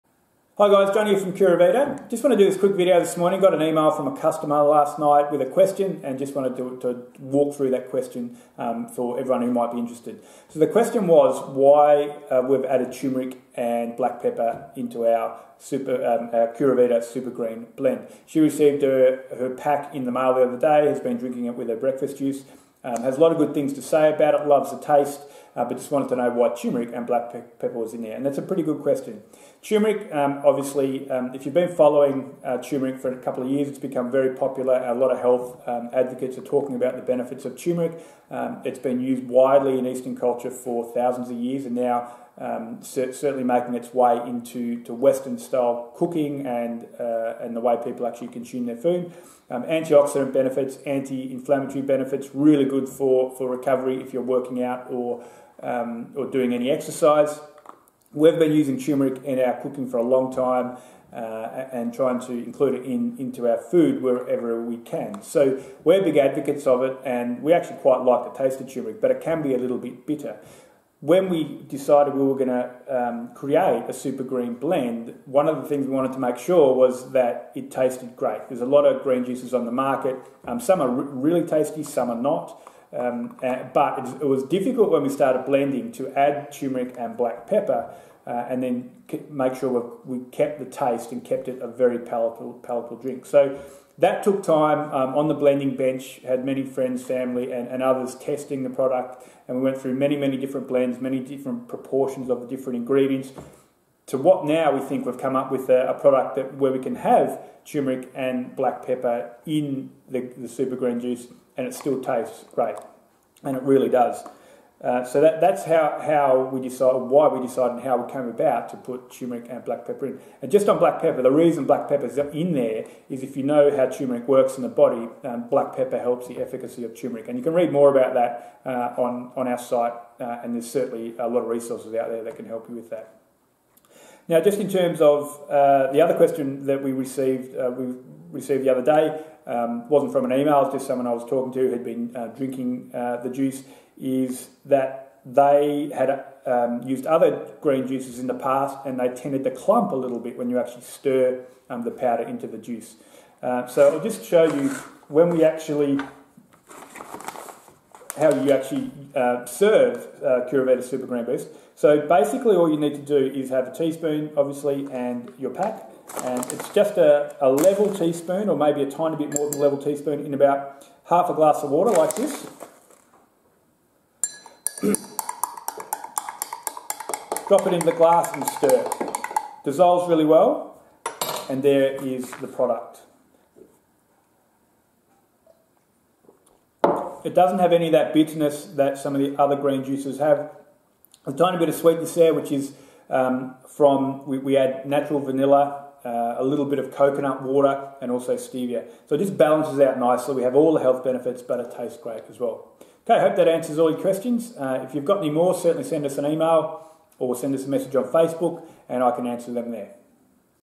Super Green Powder Question - Question From Customer On Why We Added Tumeric
We created Curavita Green Boost Supergreens to not only be incredibly healthy but also taste great. Many on the market don't taste great, so it was out mission to makes sure we achieved this.

We had a question from a customer on why we added tumeric and black pepper. This video provides the answer.